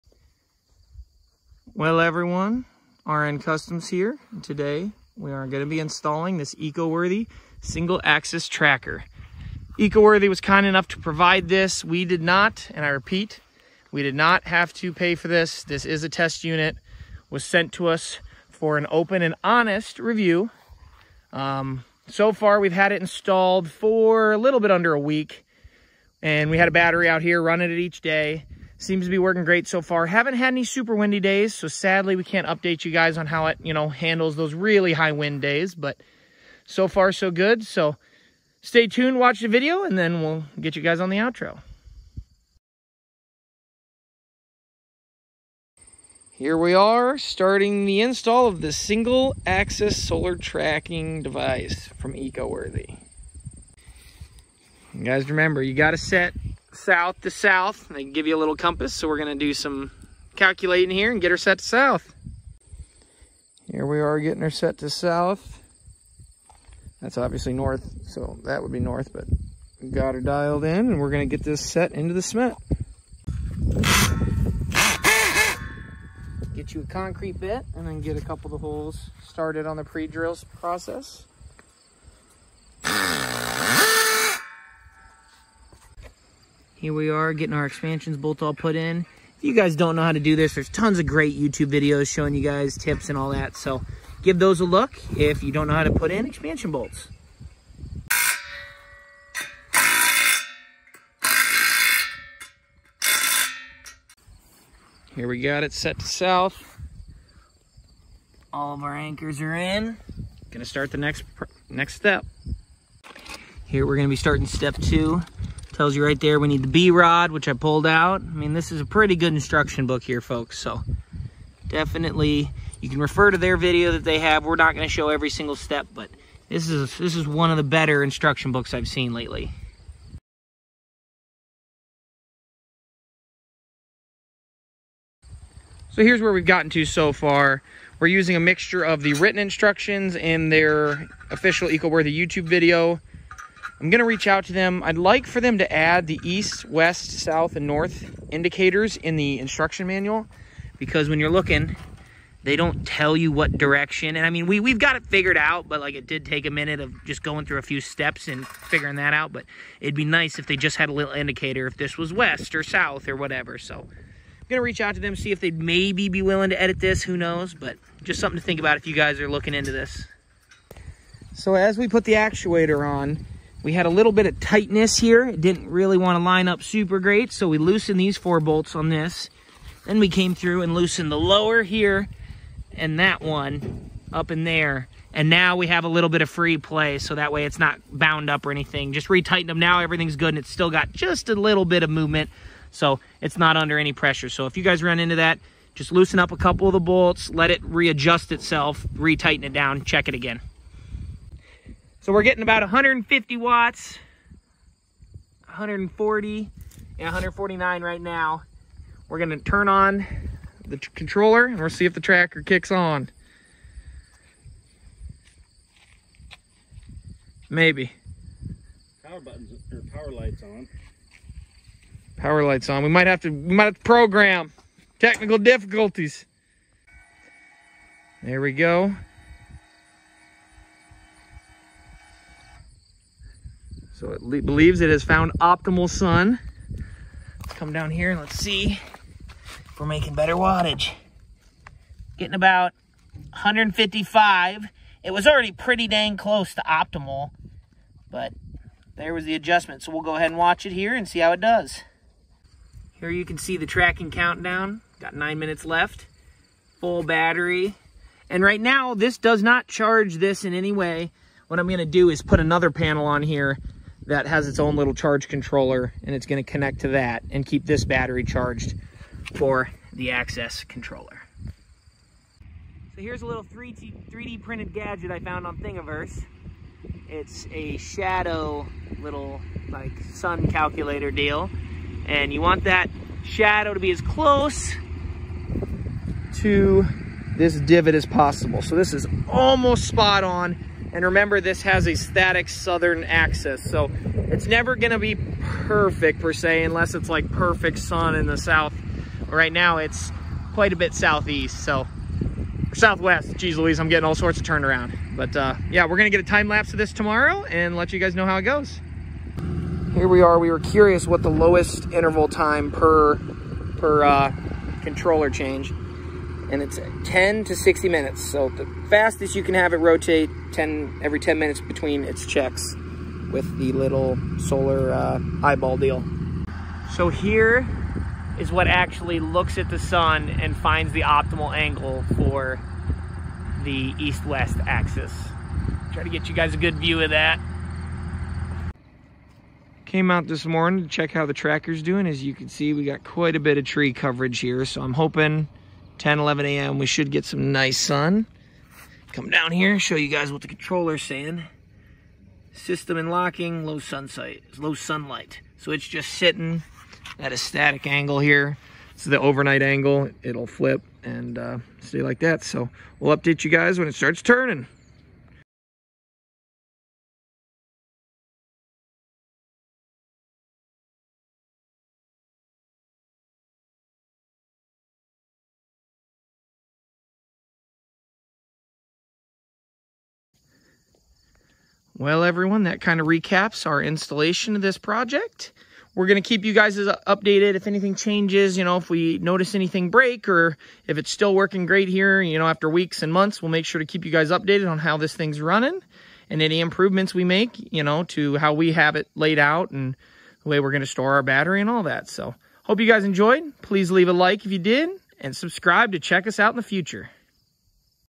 Eco Worthy Single Axis Solar Tracker Install/Review
Today we are testing out the new Eco Worthy single axis solar tracker. We plan to have more videos testing this unit out in the future.

If there are any specific tests on this solar axis tracker you want us to attempt please let us know.

If you are interested in the product, you can view it directly on the eco worthy website below. (I do typically order my eco-worthy products from Ebay)